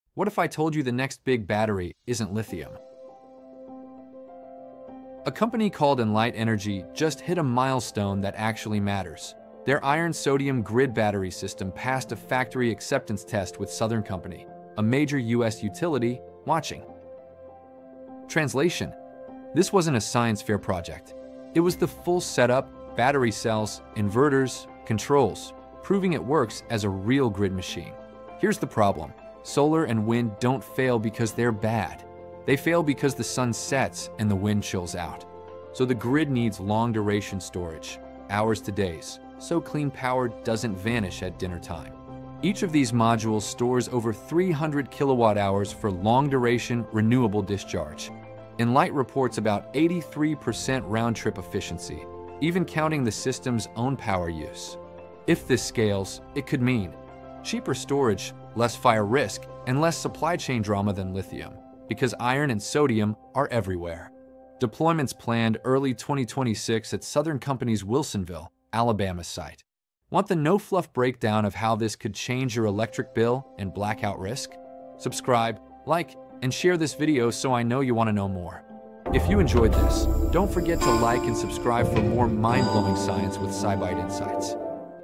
The First Field Ready Iron Sodium Battery System
Iron-sodium batteries just took a serious step out of the lab and toward the real power grid. A grid-scale iron-sodium energy storage system from Inlyte Energy completed a factory acceptance test (FAT) observed by Southern Company—meaning the full system (cells + inverters + controls) performed as expected and is ready for initial field deployment.
In this video, we break down what “field-ready” actually means, why long-duration storage is the missing piece for renewables, and how iron-sodium could compete with lithium-ion on safety, cost, and supply chain resilience.
Breakdown (key info)
What happened: Inlyte Energy completed a factory acceptance test of its first full-scale iron-sodium battery storage system
Observed by: Southern Company (major US utility)
Chemistry: sodium metal chloride + iron (iron-sodium approach)
Use case: long-duration grid storage (hours to days) where cost, stability, and lifetime matter more than energy density
Reported performance: 83% round-trip efficiency (including auxiliary loads)
What’s next: first field installs planned at Southern Company’s Energy Storage Test Site in Wilsonville, Alabama (early 2026)
Origin article (full link, no hyperlink)